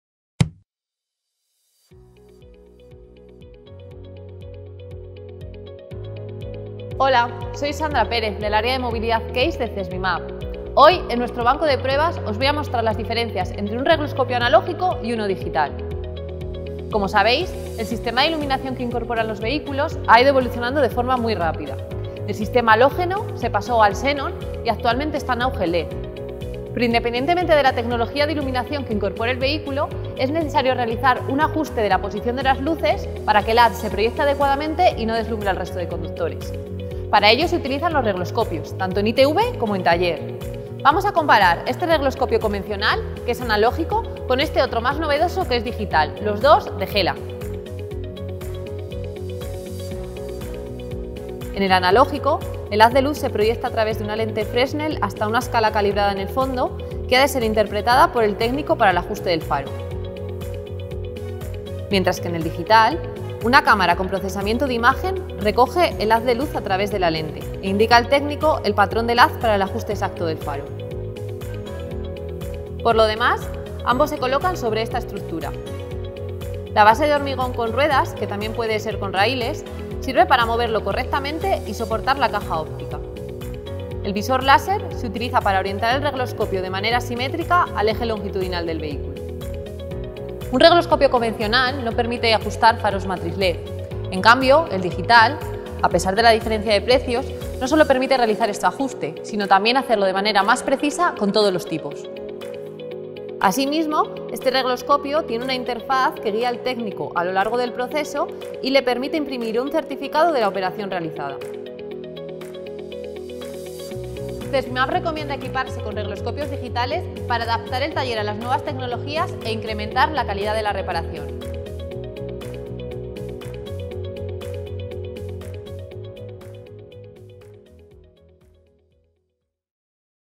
💡💡¿Qué REGLOSCOPIO elijo? ANALÓGICO o DIGITAL. Prueba con equipo HELLA 2021.
Independientemente de la tecnología de iluminación que utilice cada vehículo (halógena, xenon, led...) es necesario ajustar las luces para que el haz de luz se proyecte adecuadamente sin deslumbrar.
En el bancodePruebasCESVIMAP analizamos el regloscopio analógico vs digital HELLA (para ajustar faros matriciales)